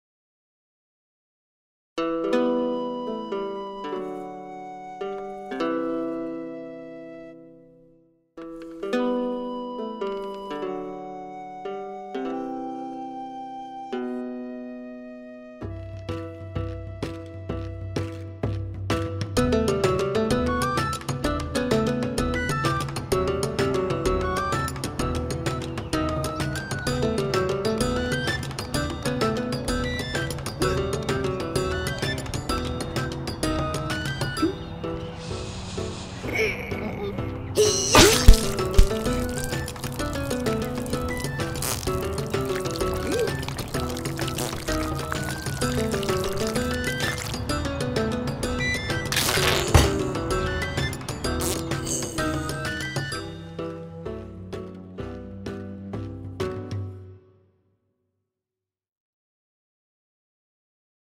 Caput! - 5 Second Day Animation 2020
A short animated medieval execution.

"By Law the herby accused is condemned to die. He dared to steal the queen's undergarments. Lord have mercy on thy soul. Off with his head" .

By Carlos Rossell.